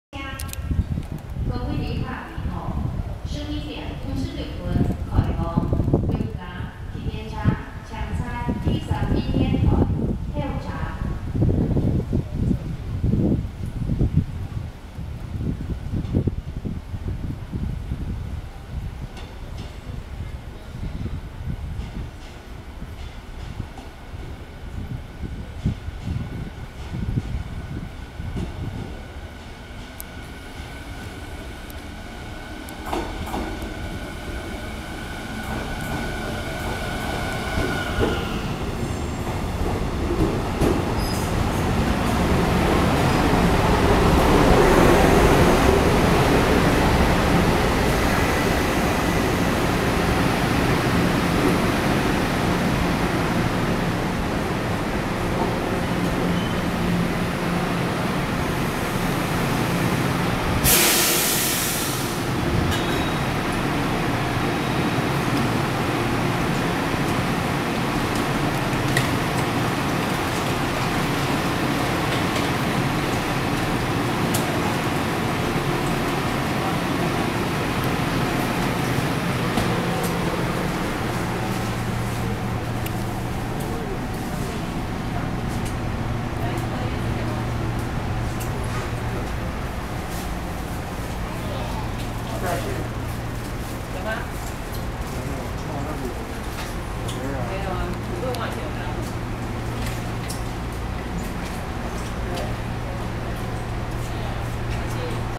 |交通迷天地| [台灣鐵路] EMU600型(新竹動物園彩繪列車) 往新竹 1731次 區間車 新竹進站
拍攝地點: 新竹站
由六家 開往新竹 1731次
沿途停靠:
六家~新竹 沿途各站

運用編組: EMU603
車況:待發